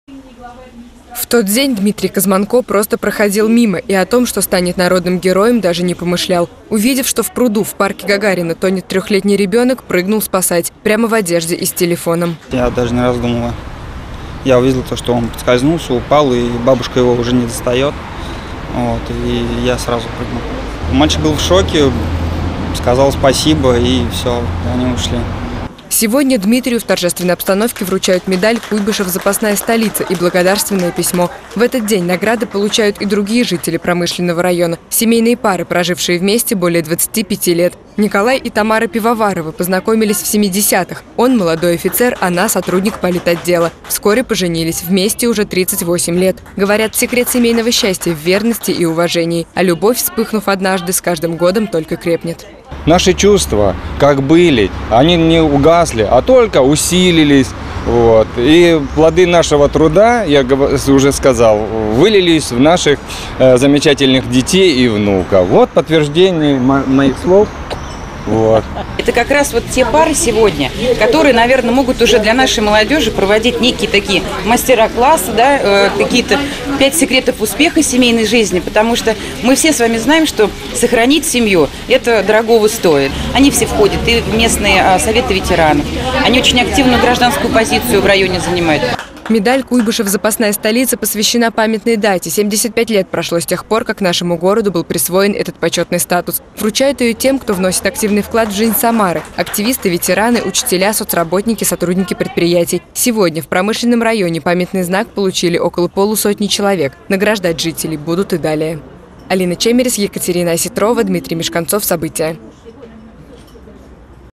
Памятные знаки "Куйбышев – запасная столица" вручили жителям Промышленного района
Медаль "Куйбышев – запасная столица" посвящена памятной дате — 75 лет прошло с тех пор, как нашему городу был присвоен этот почетный статус.